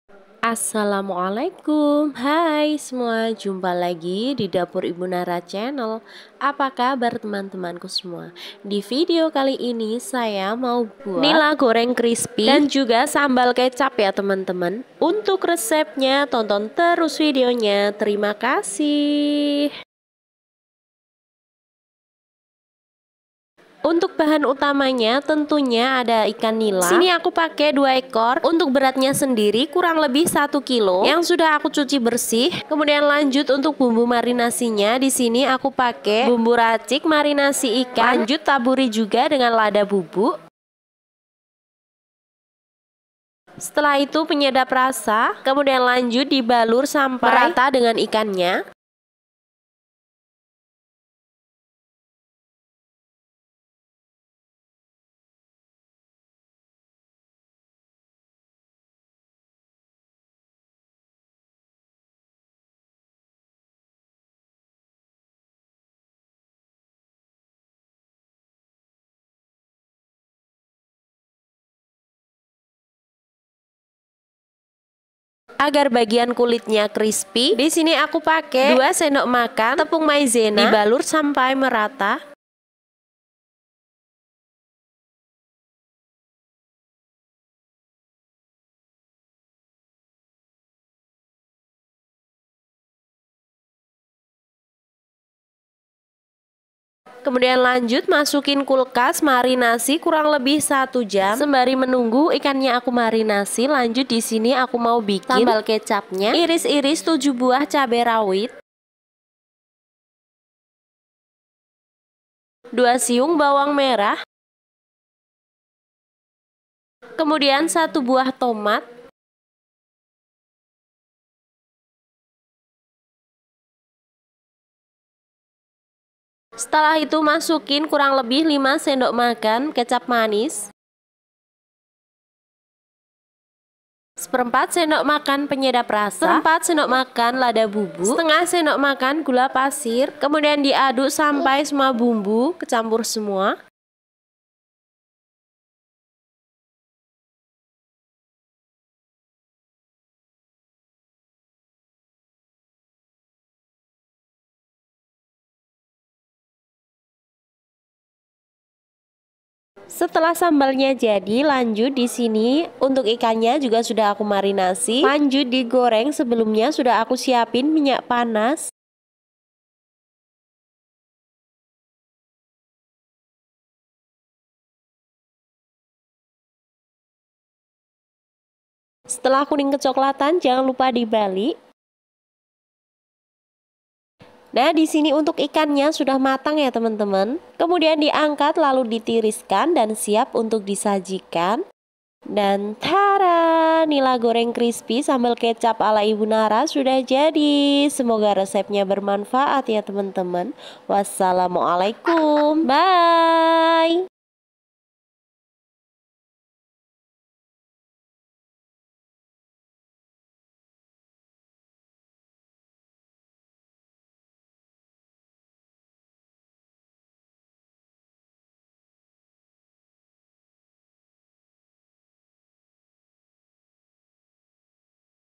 NILA GORENG KRISPI SAMBAL KECAP PALING NIKMAT DI MAKAN DENGAN NASI HANGAT‼️
BAHAN-BAHAN

- 1kg ikan nila

bumbu marinasi
- Indofood bumbu marinasi ikan
- lada bubuk
- penyedap rasa
- 2sdm tepung maizena

sambal kecap
- 7 buah cabe rawit
- 2 siung bawang merah
- 1 buah tomat
- 5sdm kecap manis
- 1/4sdm penyedap rasa
- 1/4sdm lada bubuk
- 1/2sdm gula pasir